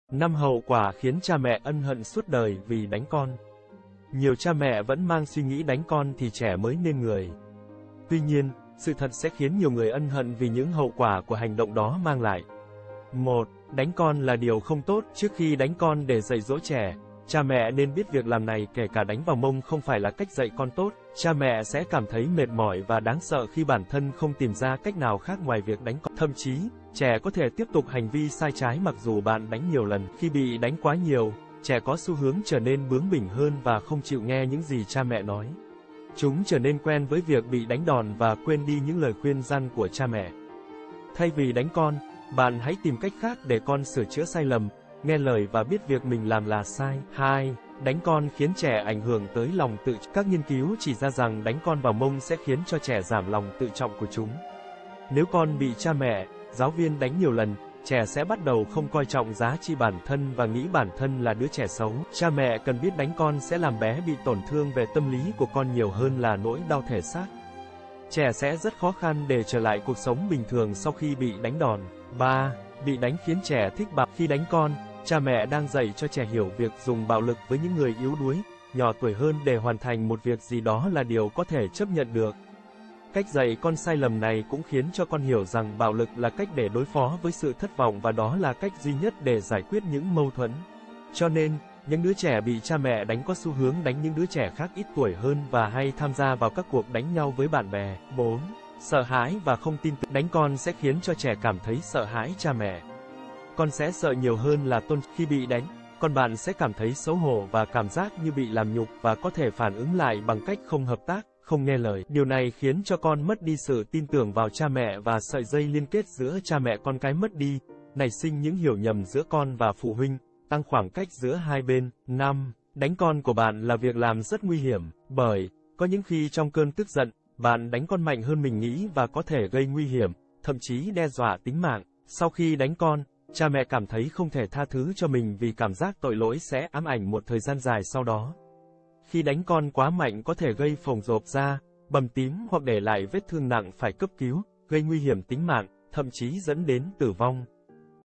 5 hậu quả khiến cha mẹ ân hận suốt đời vì đánh con
5 hậu quả khiến cha mẹ ân hận suốt đời vì đánh con
Nhiều cha mẹ vẫn mang suy nghĩ đánh con thì trẻ mới nên người. Tuy nhiên, sự thật sẽ khiến nhiều người ân hận vì những hậu quả của hành động đó mang lại.